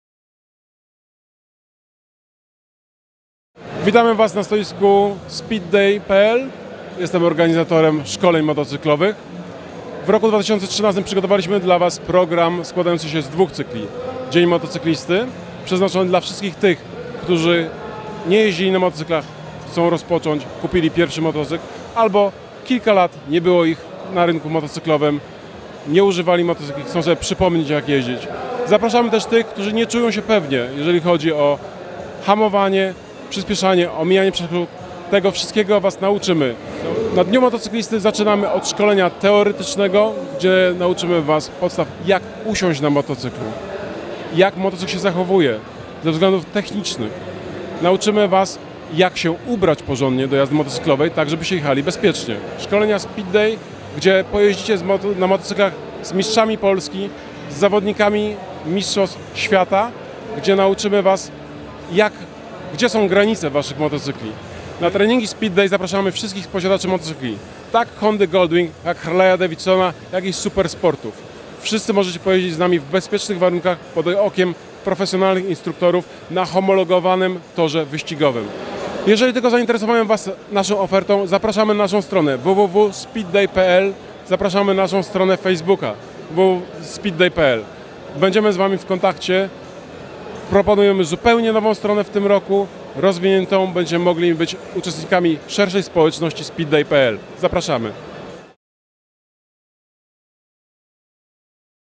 Wywiad z Arturem Urbańskim - SPEED DAY
Wywiad z Arturem Urbańskim ze SPEED DAY podczas Wrocław Motorcycle Show 2013